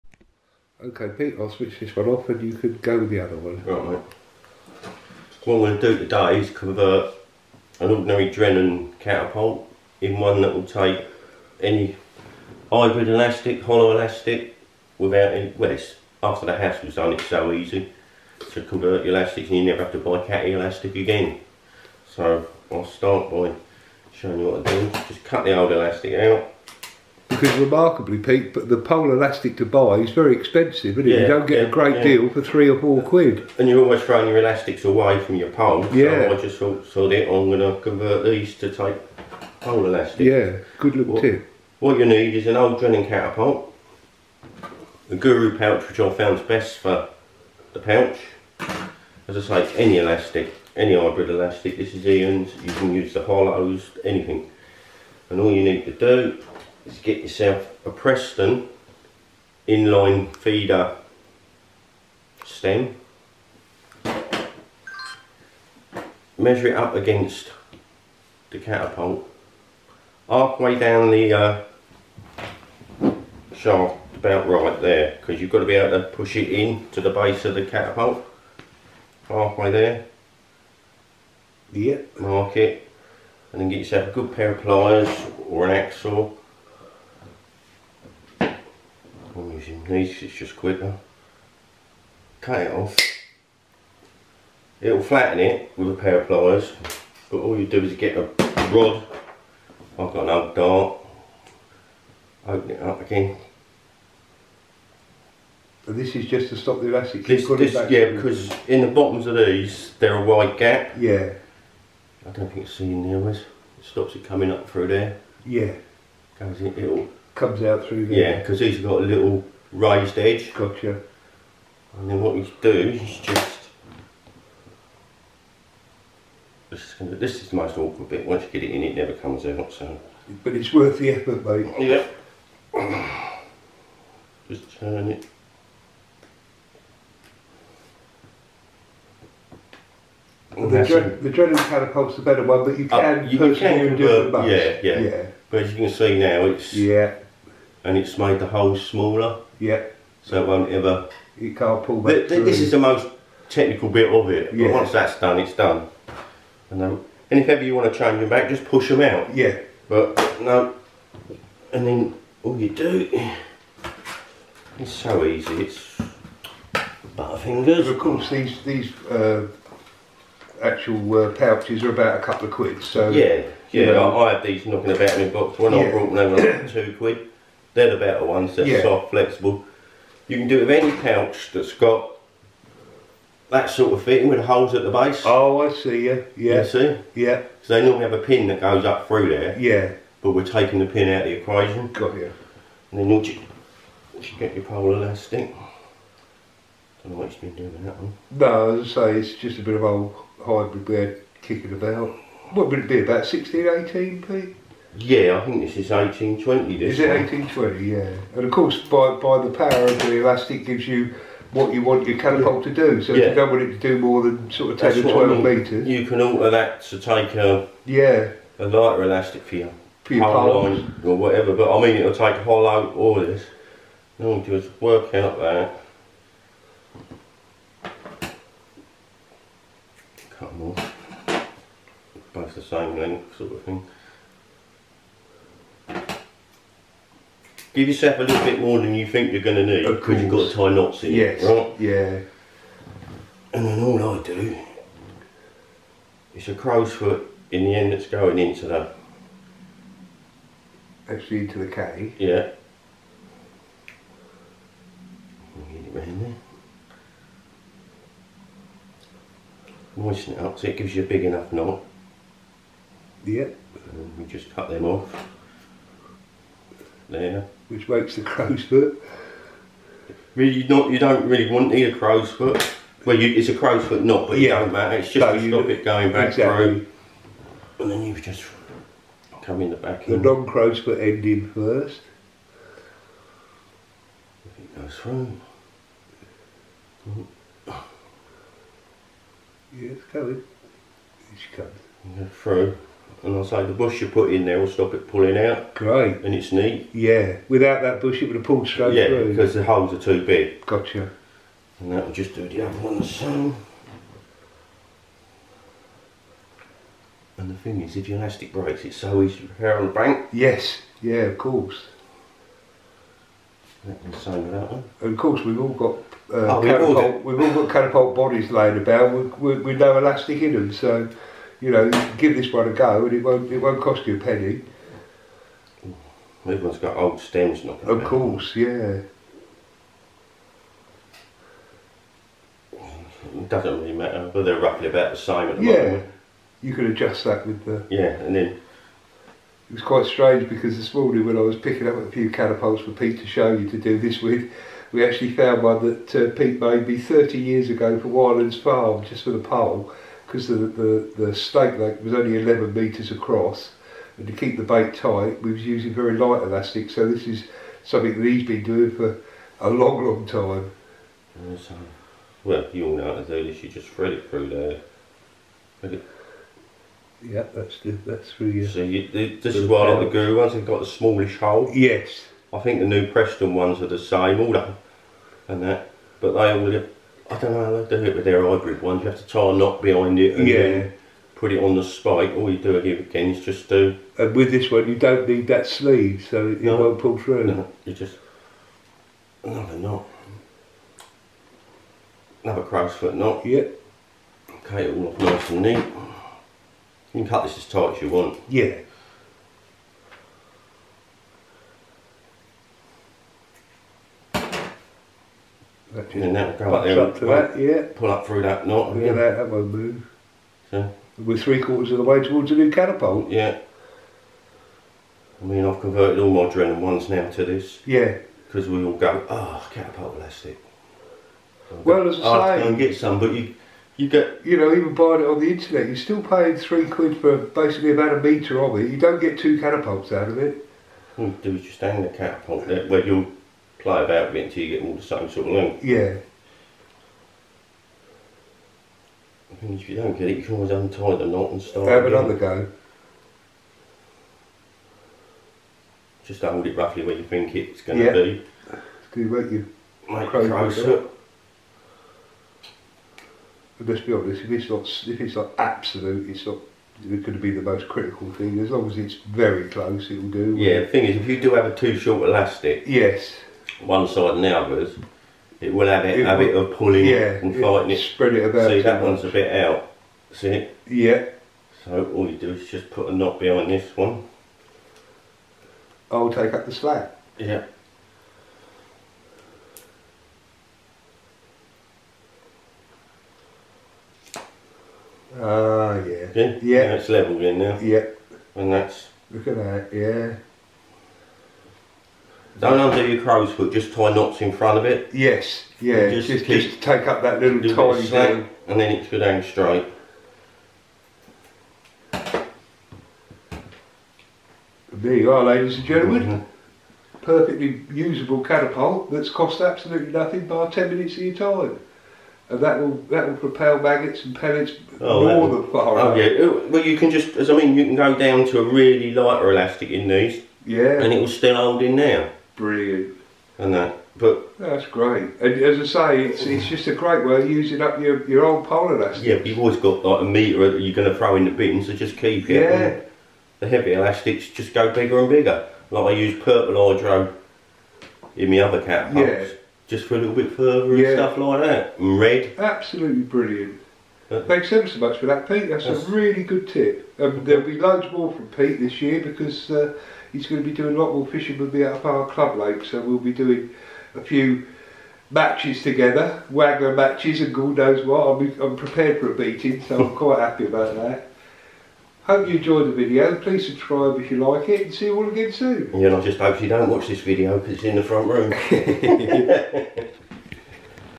Hints and tips with Peter Joiner (match angler)
In this video we see Peter using up old pole elastic to make bespoke catapults for all situations.Close up shots in video two will show you how to make these catapults and save money!